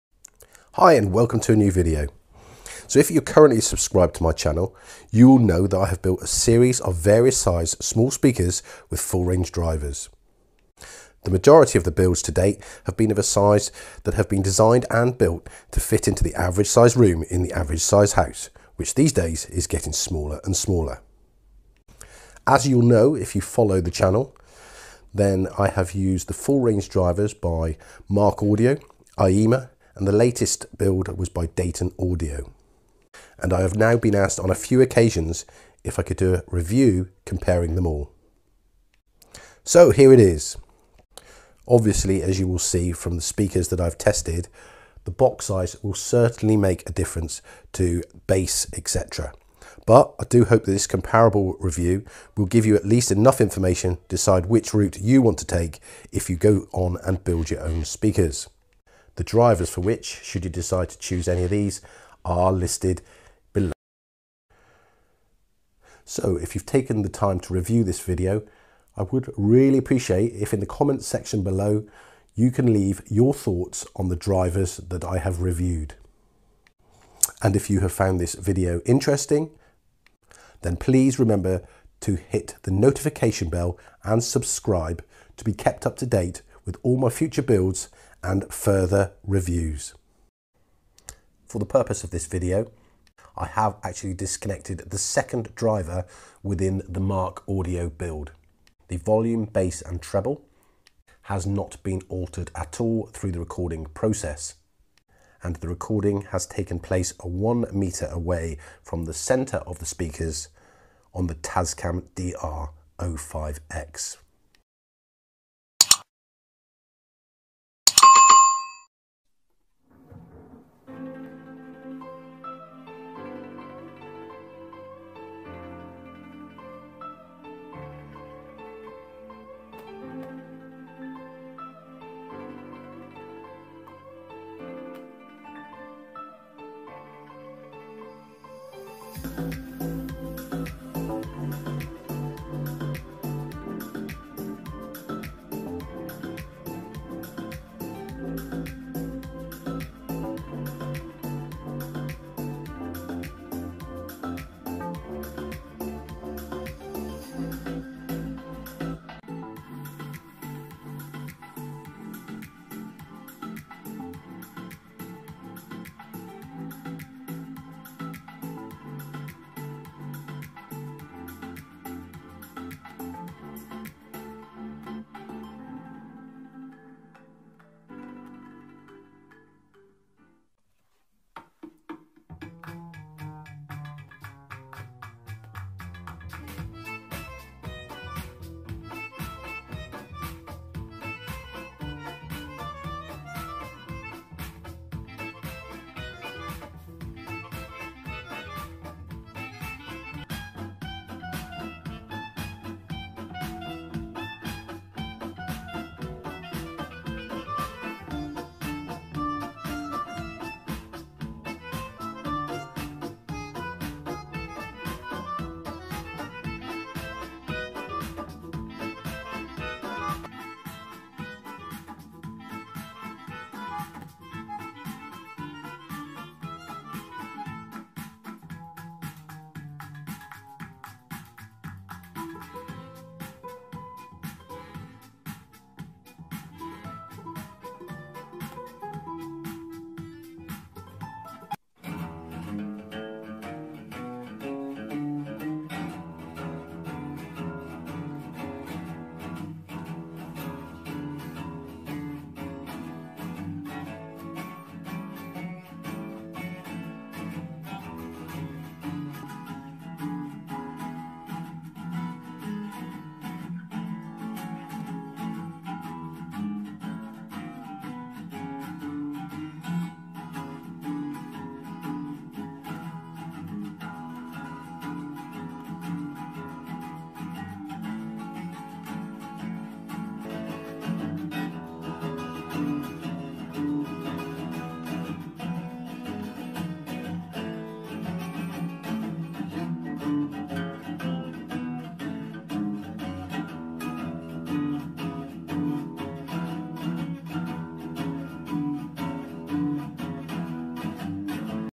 Dayton Audio PS95-8 v Markaudio CHN-50 v AIYIMA 3" Drivers Compared. Best Small Full Range Driver?
Which is Best? Dayton Audio PS95-8 or Markaudio CHN-50 or AIYIMA 3" Full Range Drivers. I have tried to put together a short review of all 3. The box sizes do differ though so, you will need to take that into consideration. The driver for the Markaudio CHN-50 was configured to use just the single driver for the review.

AIYIMA FULL RANGE

Bluetooth Amplifier: Buy from China and Save £30/$30/€30
FOSI BT20A AMP